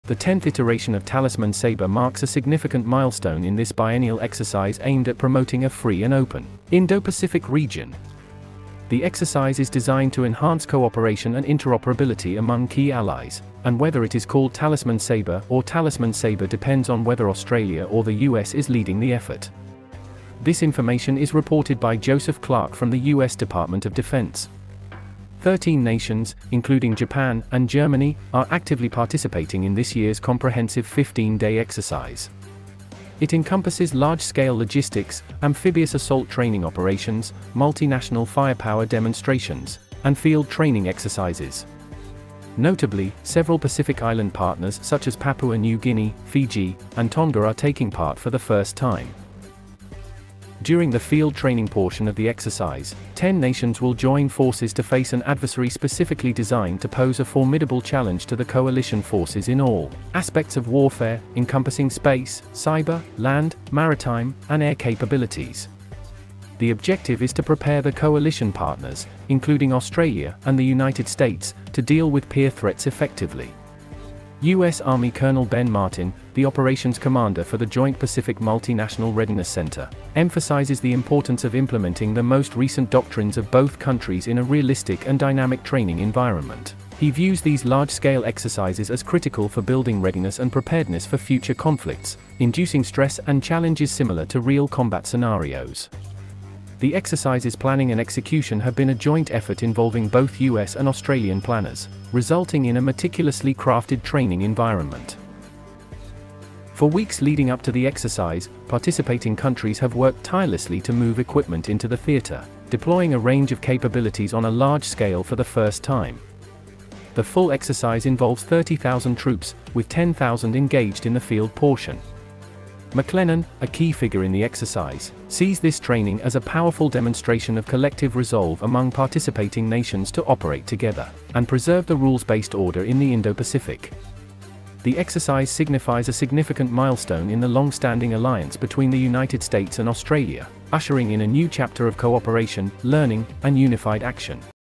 Talisman Sabre 23 Field Exercise in Australia inaugurates new era for combined military training in
Talisman Sabre 23 Field Exercise in Australia inaugurates new era for combined military training in Indo-Pacific